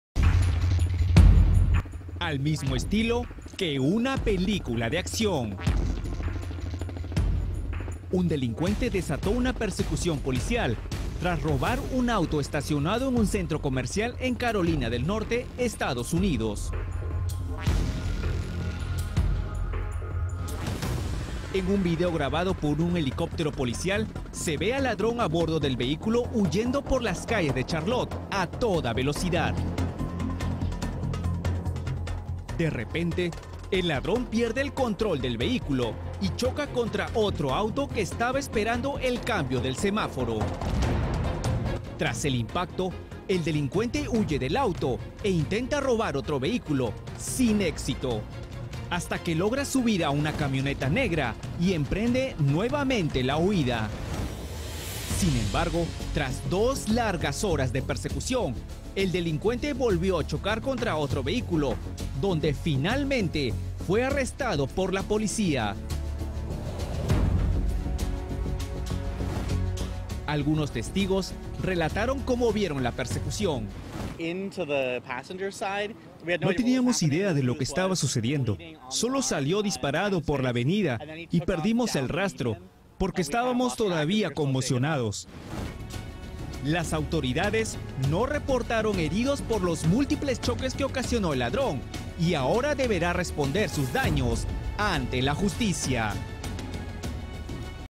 Estados Unidos: ladrón roba auto y protagoniza espectacular persecución policial
Un delincuente desató una persecución policial tras robar una camioneta estacionada en un centro comercial en Carolina del Norte, Estados Unidos.